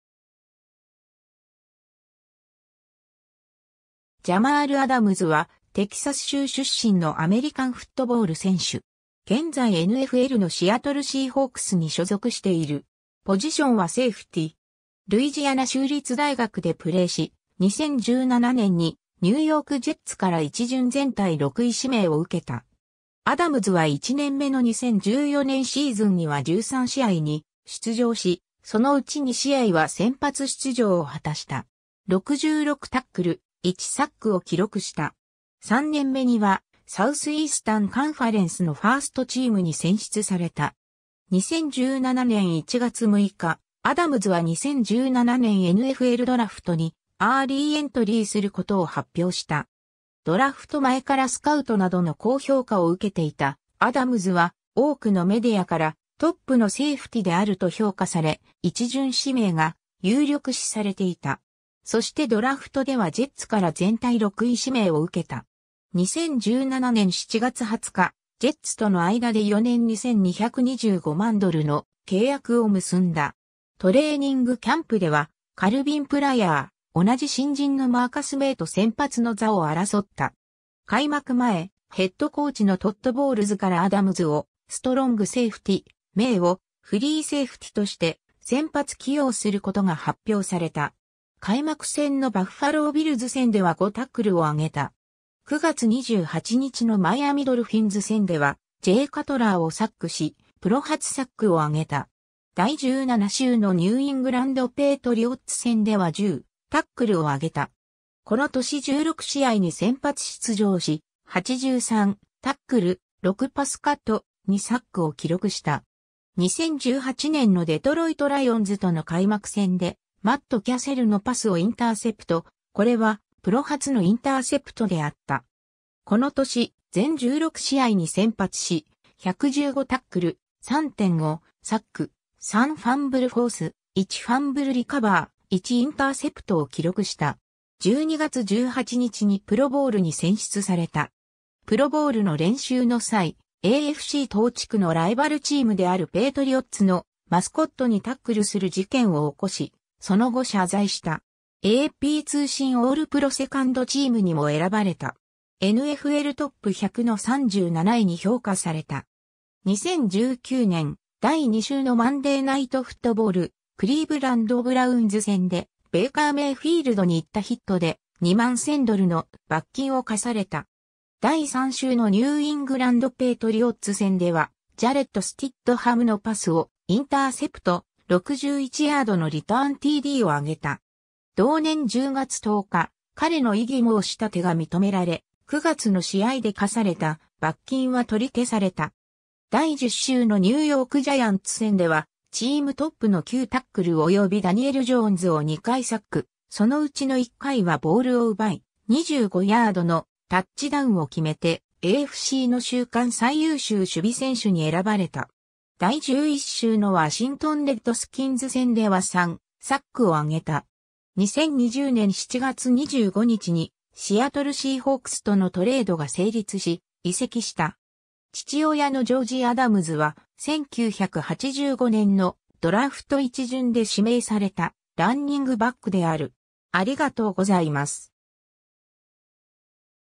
ジャマール・アダムズ
ニューヨーク・ジェッツの選手
シアトル・シーホークスの選手
ジャマール・アダムズ（Jamal Lee Adams、1995年10月17日 - ）はテキサス州出身のアメリカンフットボール選手。
現在NFLのシアトル・シーホークスに所属している。
ポジションはセイフティ。
ルイジアナ州立大学でプレーし、2017年にニューヨーク・ジェッツから1巡全体6位指名を受けた。
 アダムズは1年目の2014年シーズンには13試合に出場し、そのうち2試合は先発出場を果たした。
66タックル、1サックを記録した。
3年目には、サウスイースタン・カンファレンスのファースト・チームに選出された。
 2017年1月6日、アダムズは2017年NFLドラフトにアーリーエントリーすることを発表した。
 ドラフト前からスカウトなどの高評価を受けていたアダムズは、多くのメディアからトップのセイフティであると評価され、1巡指名が有力視されていた。
そしてドラフトではジェッツから全体6位指名を受けた。
 2017年7月20日、ジェッツとの間で4年2225万ドルの契約を結んだ。
トレーニングキャンプでは、カルビン・プライアー、同じ新人のマーカス・メイと先発の座を争った。
開幕前、ヘッドコーチのトッド・ボウルズからアダムズをストロング・セイフティ、メイをフリーセイフティとして先発起用することが発表された。
開幕戦のバッファロー・ビルズ戦では5タックルをあげた。
9月28日のマイアミ・ドルフィンズ戦ではジェイ・カトラーをサックし、プロ初サックをあげた。
第17週のニューイングランド・ペイトリオッツ戦では10タックルをあげた。
この年16試合に先発出場し、83タックル、6パスカット、2サックを記録した。
 2018年のデトロイト・ライオンズとの開幕戦でマット・キャセルのパスをインターセプト、これはプロ初のインターセプトであった。
この年全16試合に先発し、115タックル、3.5サック、3ファンブルフォース、1ファンブルリカバー、1インターセプトを記録した。
12月18日にプロボウルに選出された。
プロボウルの練習の際、AFC東地区のライバルチームであるペイトリオッツのマスコットにタックルする事件を起こし、その後謝罪した。
AP通信オールプロセカンドチームにも選ばれた。
NFL top 100の37位に評価された。
 2019年、第2週のマンデーナイトフットボール、クリーブランド・ブラウンズ戦でベイカー・メイフィールドに行ったヒットで、21000ドルの罰金を科された。
第3週のニューイングランド・ペイトリオッツ戦ではジャレット・スティッドハムのパスをインターセプト、61ヤードのリターンTDをあげた。
同年10月10日、彼の異議申立が認められ、9月の試合で科された罰金は取り消された。
第10週のニューヨーク・ジャイアンツ戦ではチームトップの9タックル及びダニエル・ジョーンズを2回サック、そのうちの1回はボールを奪い、25ヤードのタッチダウンを決めて、AFCの週間最優秀守備選手に選ばれた。
第11週のワシントン・レッドスキンズ戦では3サックをあげた。
 2020年7月25日にシアトル・シーホークスとのトレードが成立し、移籍した。
 父親のジョージ・アダムズは、1985年のドラフト1巡で指名されたランニングバックである。